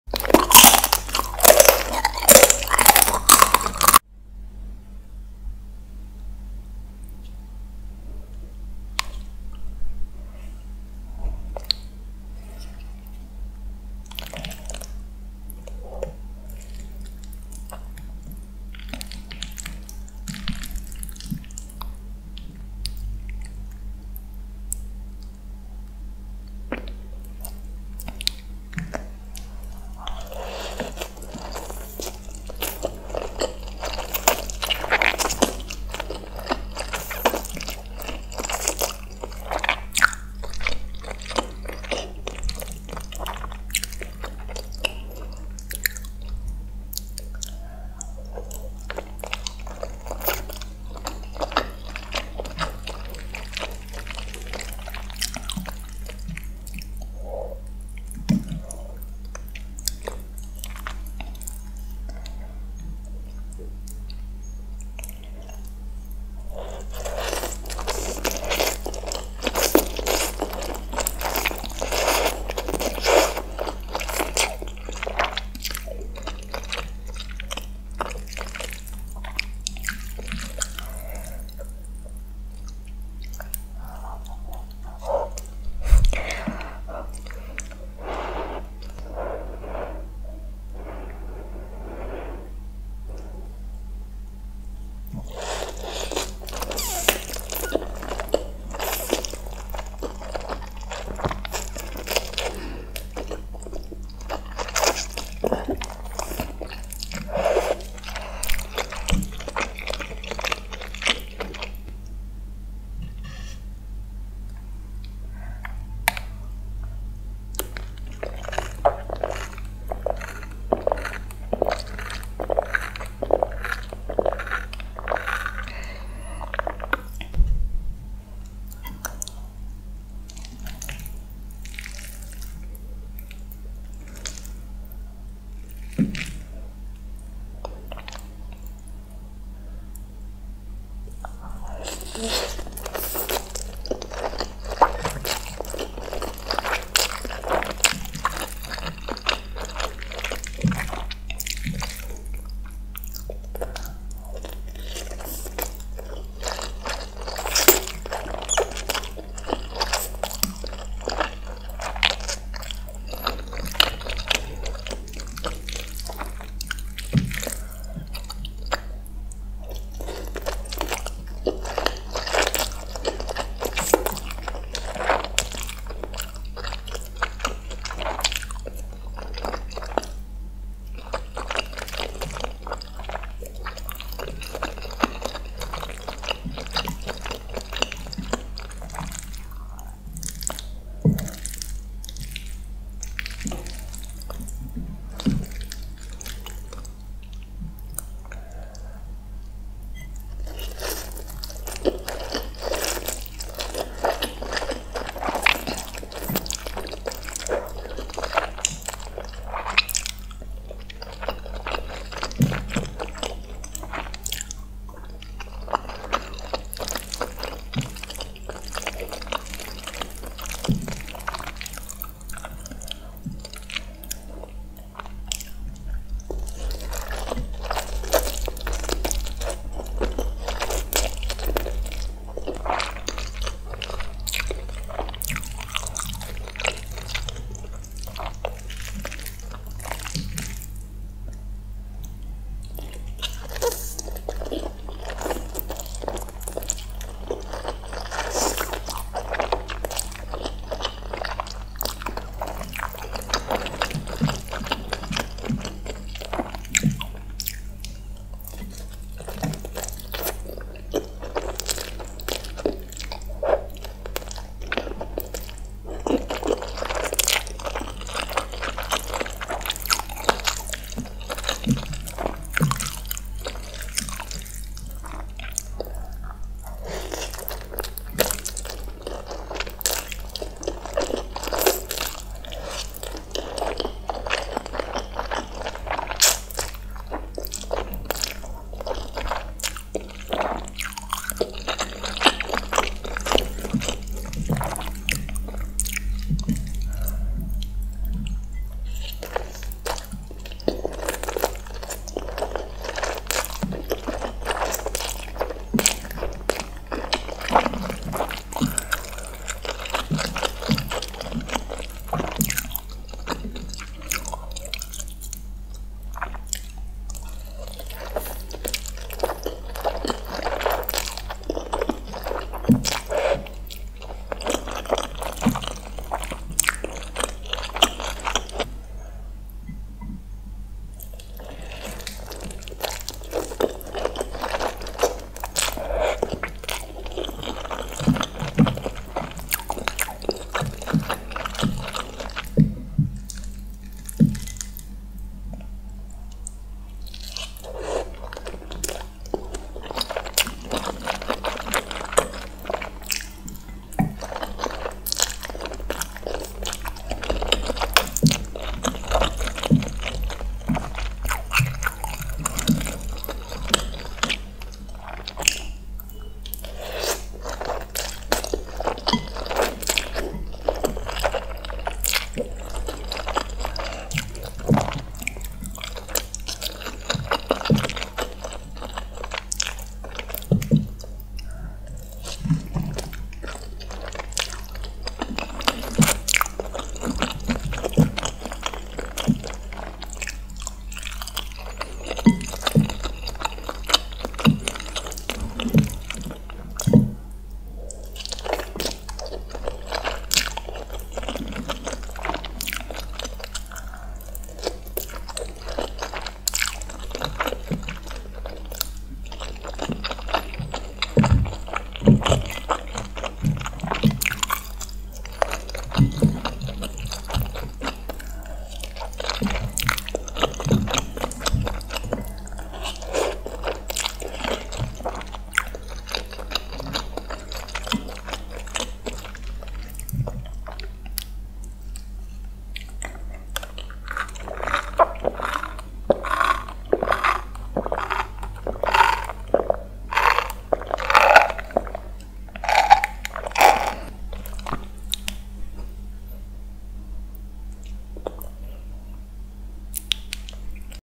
ASMR SPAGHETTI WITH SAUSAGE & GUAVA JUICE (Request) | EATING SOUNDS | ASMR INDONESIA | NO TALKING
Food 🍽
Chicken Spaghetti with Sausage

Equipment :
- Blue Yeti🎤

Yang mau request, silahkan tinggalkan pesan di kolom komentar.